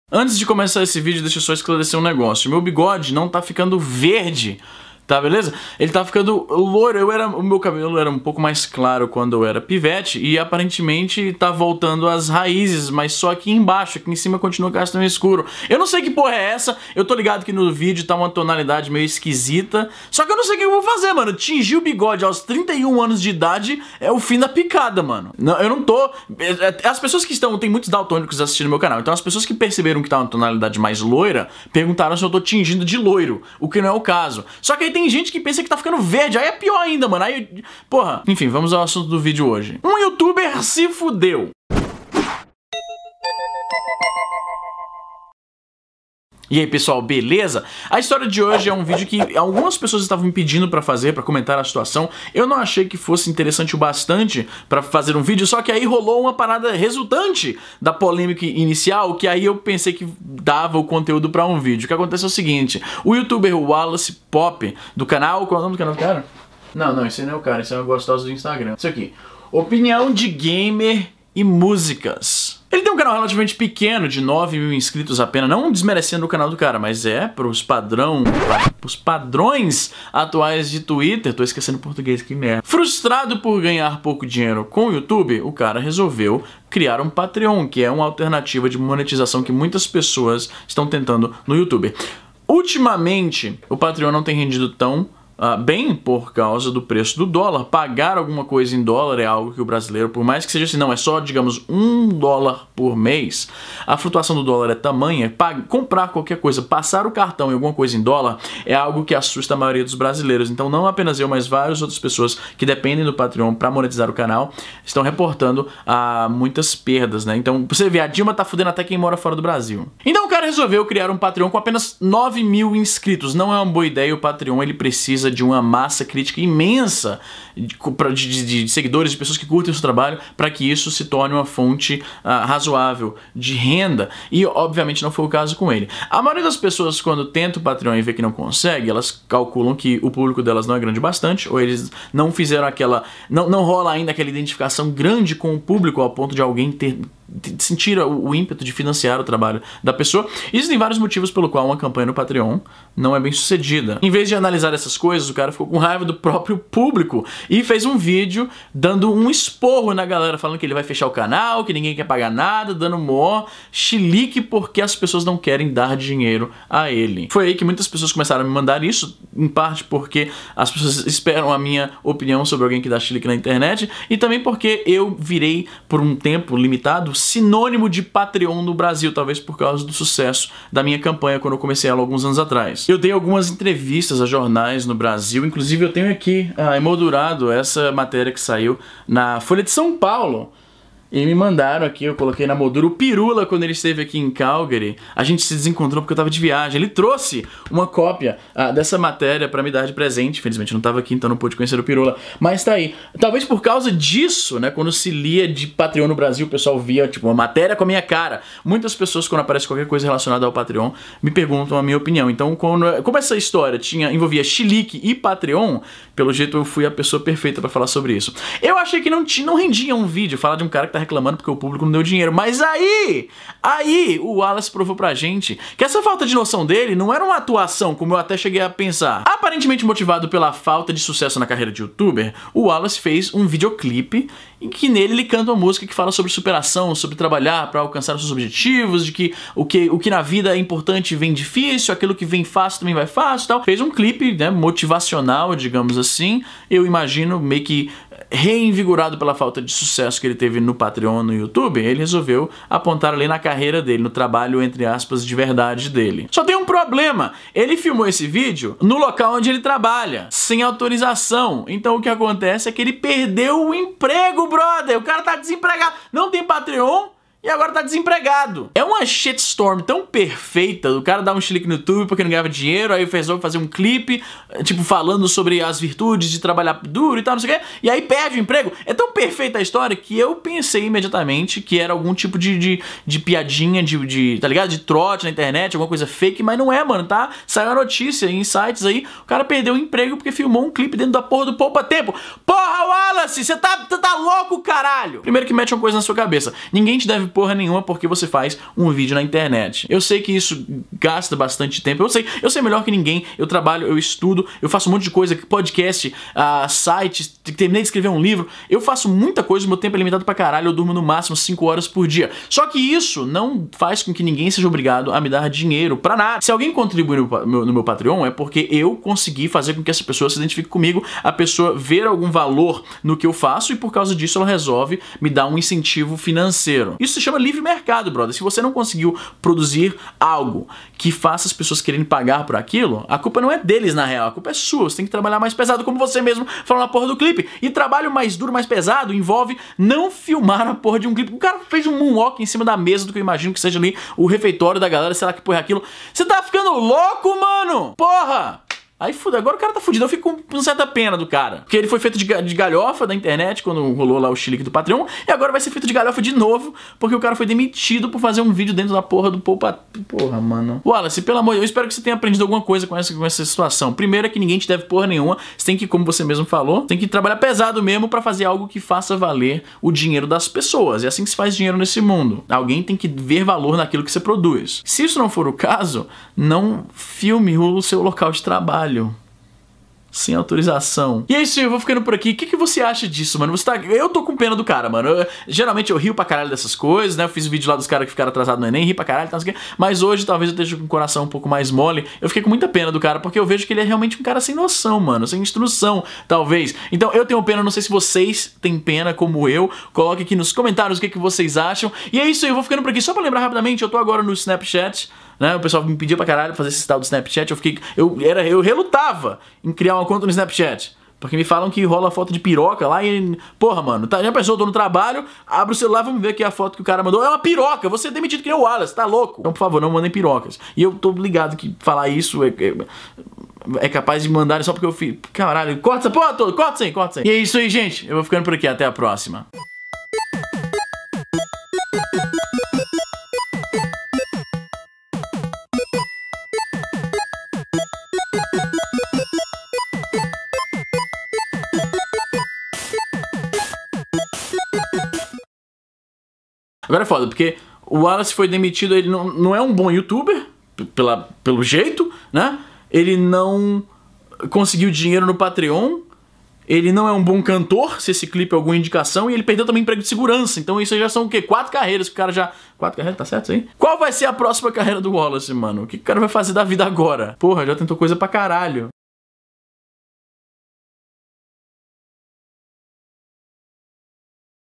Justa causa por causa de um vídeo?!
Youtuber perde emprego por causa de clipe

As aberturas do canal são feitas pelo Murilo Almeida. Prestigiem o trabalho dele: